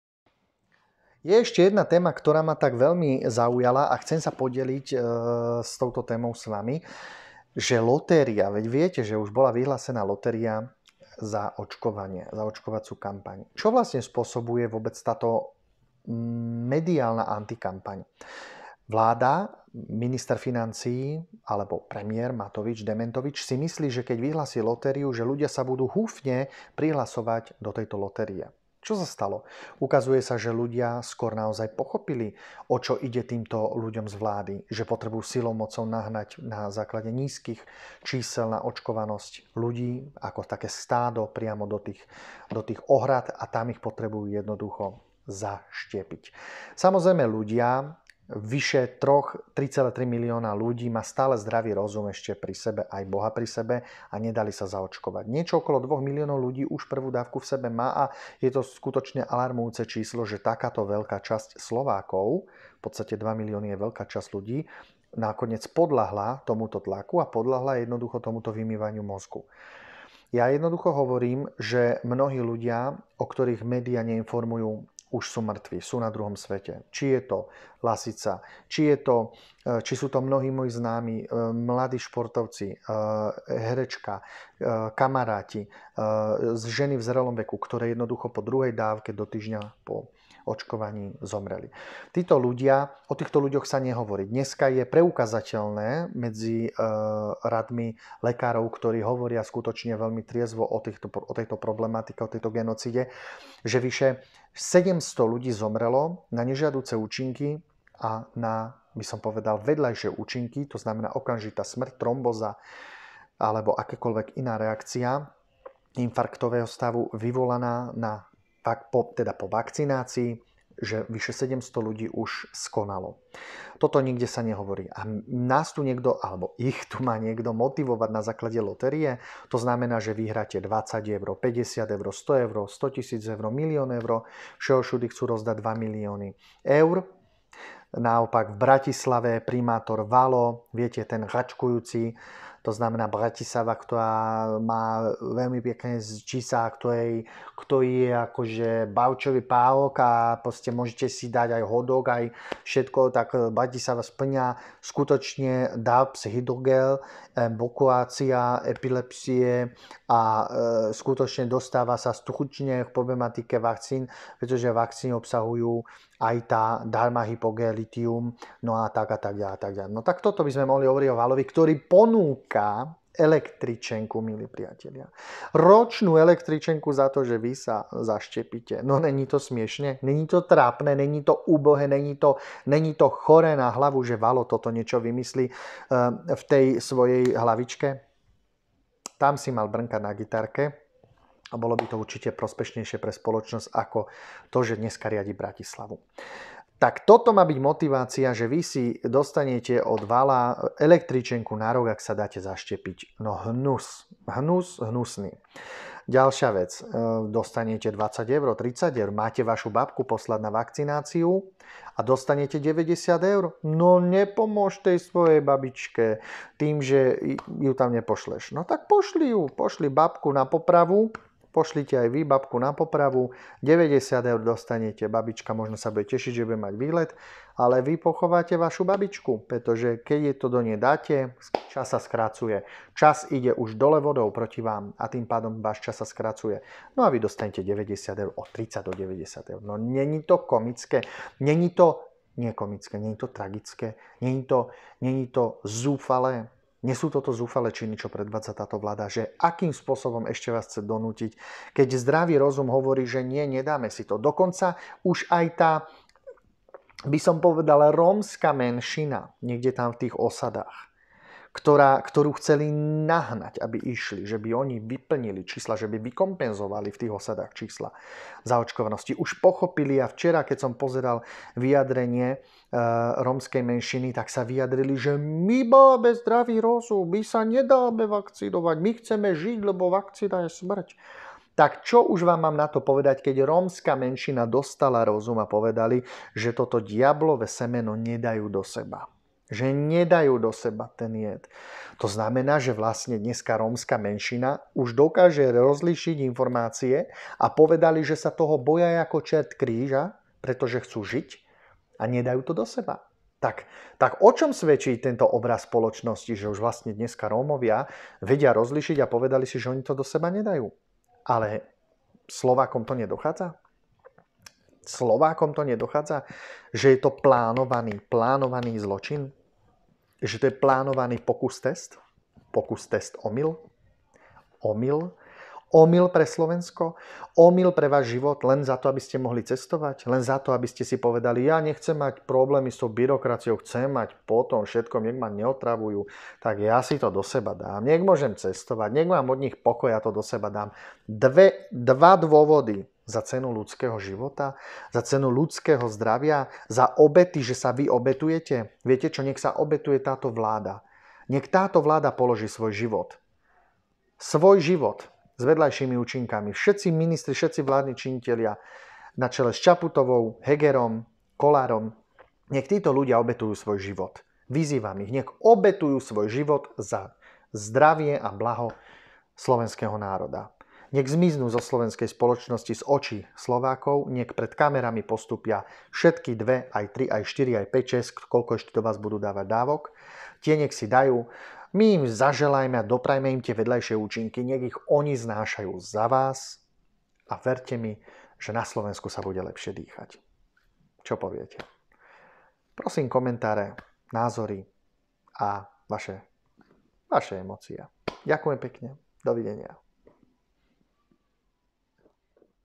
Jakubec: Za zaočkovanie Lotéria od dementoviča a električenka od račkára z mesta.
Tomu sa povie terno. Výhra od blbcov, ktorá má ohúriť a motivovať. Možno istú skupinku hlupákov, rozhodne však demotivuje.
Zdravý človek s rozumom ani len nepomyslí nad niečim takým.
A vďaka zúfalému rozhodnutiu sa približujete na hrobové miesto.